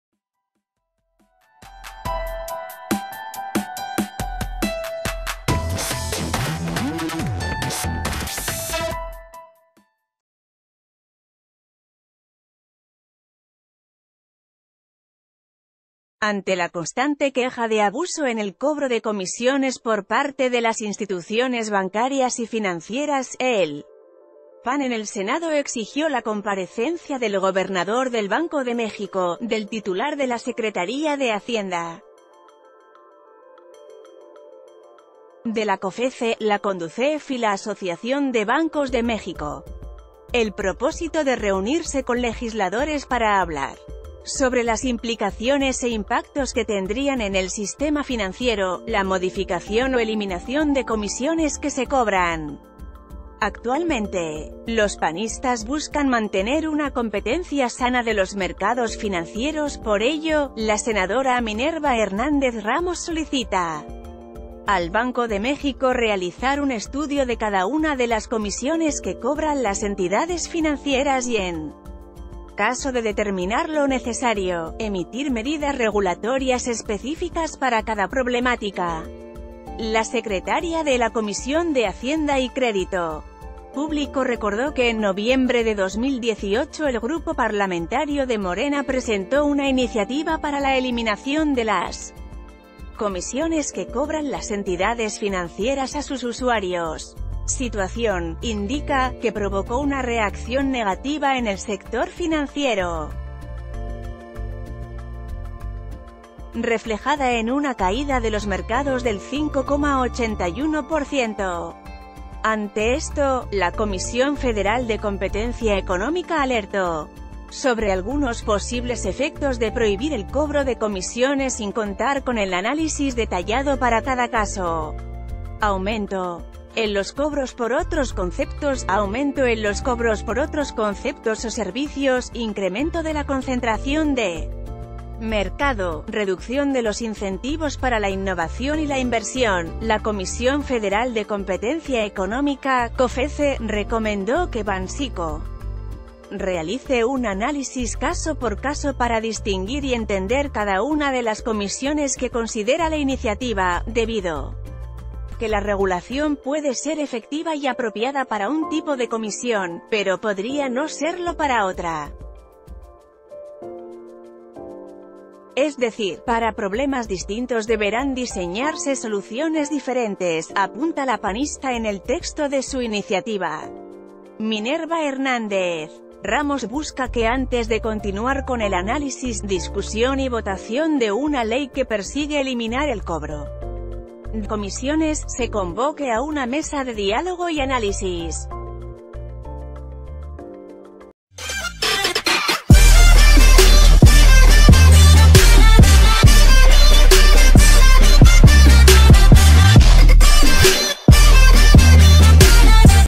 PAN pide comparecencias de titulares de Banxico, Cofece y Condusef por comisiones bancarias
Ante la constante queja de abuso en el cobro de comisiones por parte de las instituciones bancarias y financieras, el PAN en el Senado exigió la comparecencia del gobernador del Banco de México, del titular de la Secretaría de Hacienda, de la Cofece, la Conducef y la Asociación de Bancos de México. ...